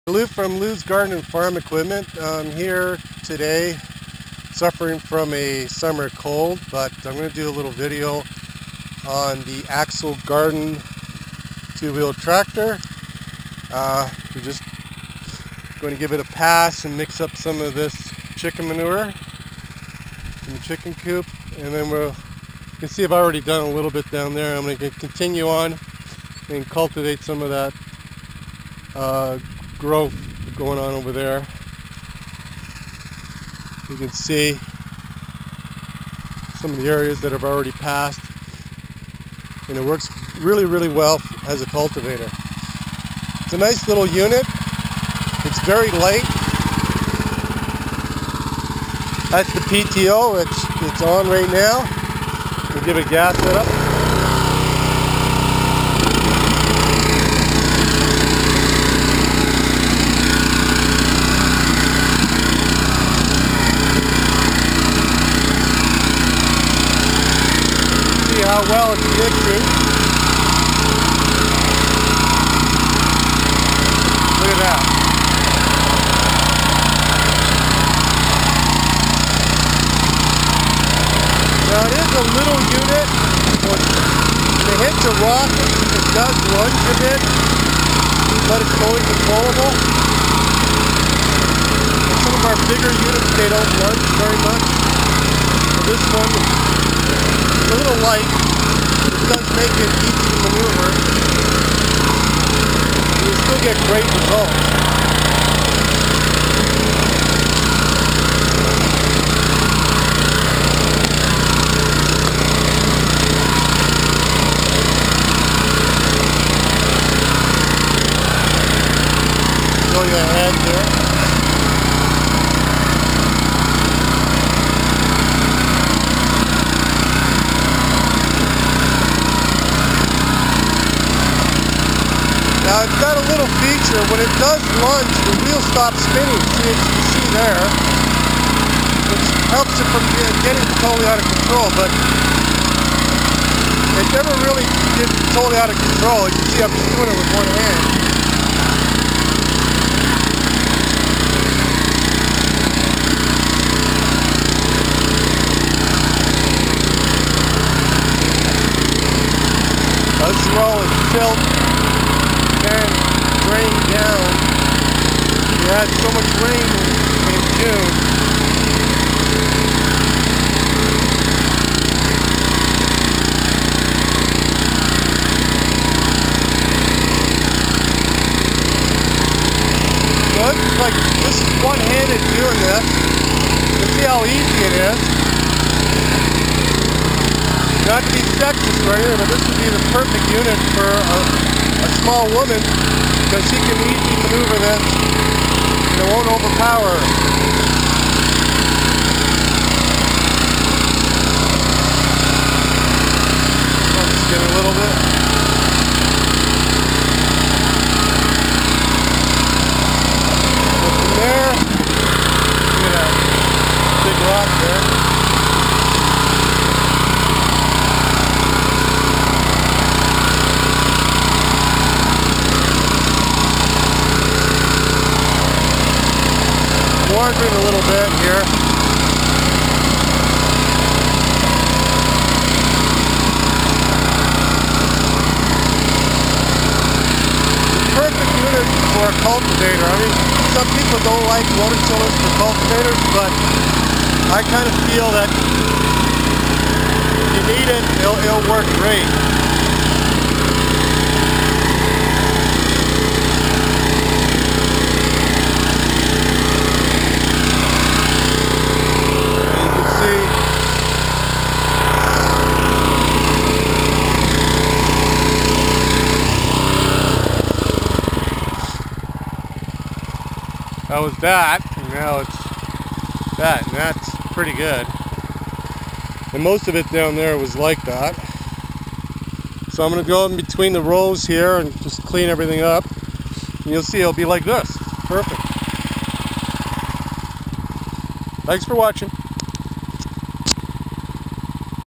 AXO Garden 2 Wheeled Tractor in Action
Following up a AXO Garden review with a field test.
The AXO Garden Gerky, is a joy, very light easy to handle, as a cultivator can get very close to standing plants.As a rototiller Idea for small to medium gardens. Quick change pto connection.